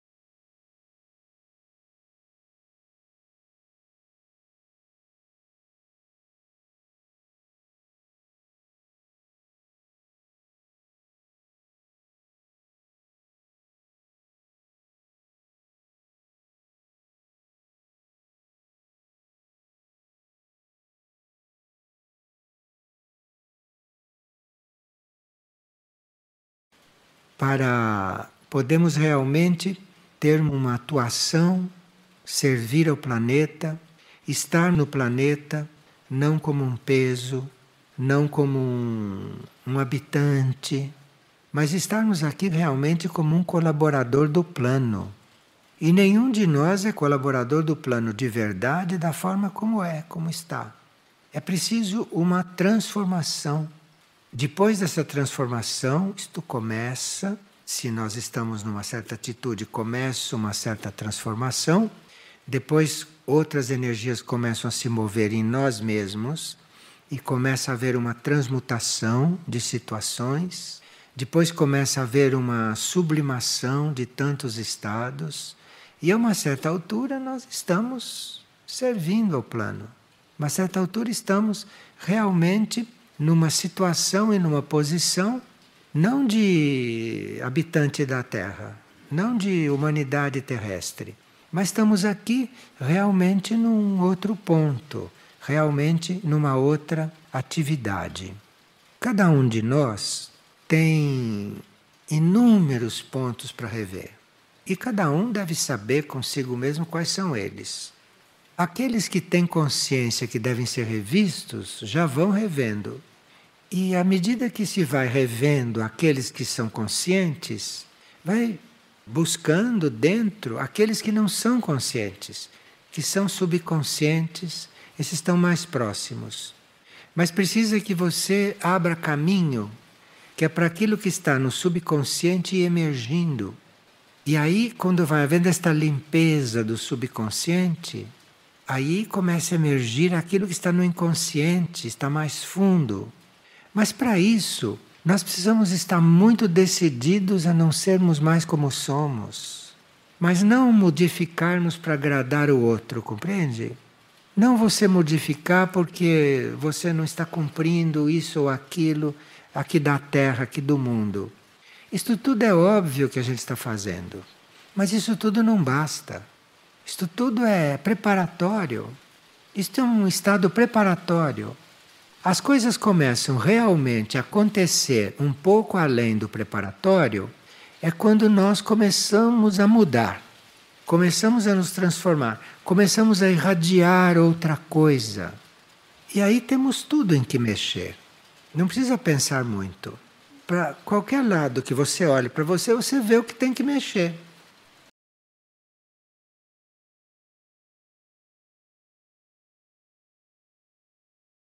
17-10-2023 | Pensamento do Dia / Thought of the Day / Pensamiento del Día | Trigueirinho
"Sois agraciados, pois buscais a transformação."

"You are graced, for you seek transformation."

"Sois agraciados, pues buscáis la transformación."

Legendas em espanhol ou inglês, clique no botão “Detalhes/Setting/Configuración”